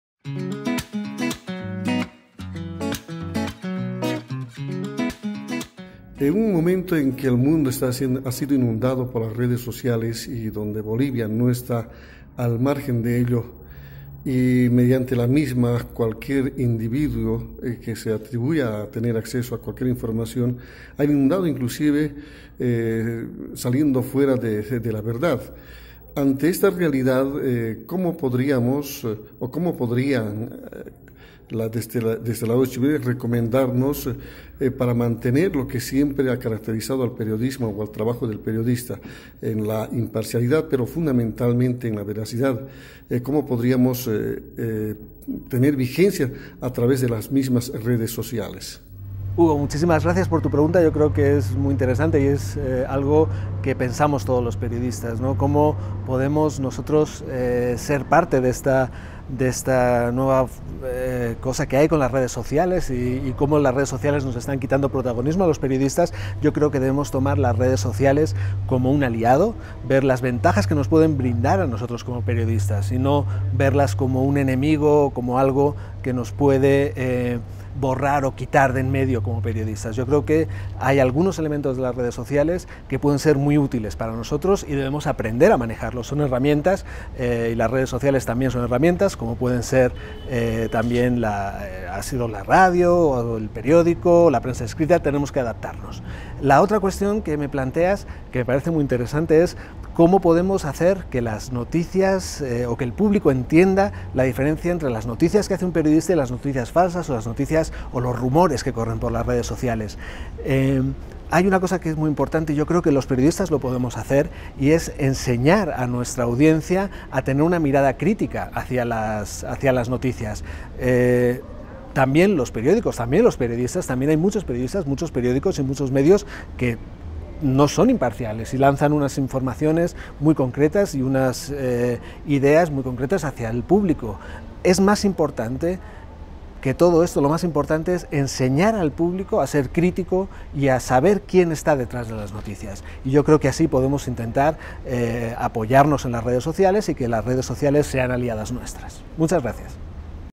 PremioRadioTaller: Hugo Serrano
Hugo Serrano de Radio Encuentro de Punata ha ganado un premioradiotaller.

Ve la respuesta en el video.

Este video es parte del "Curso de especialización en periodismo radiofónico digital" del Centro de producción radiofónica (CEPRA) para Radios Comunitarios y RPO.